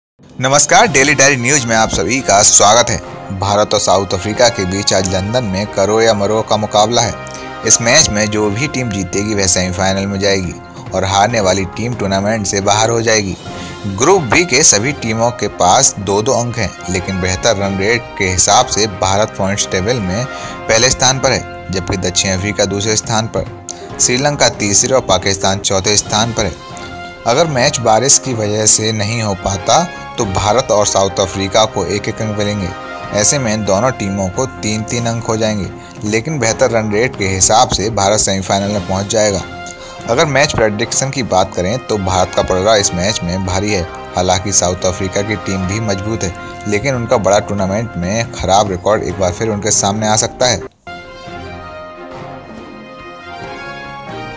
India vs South Africa || Match Prediction || ICC Champions Trophy 2017 || Match Preview
Discord and defeat, or amity and victory? Which way will perceptions sway after the virtual quarterfinal against South Africa?
Indian cricket's English summer has been a tumultuous one so far, but Sunday's knockout game also provides Virat Kohli's men a chance to maintain their excellent record in do-or-die situations.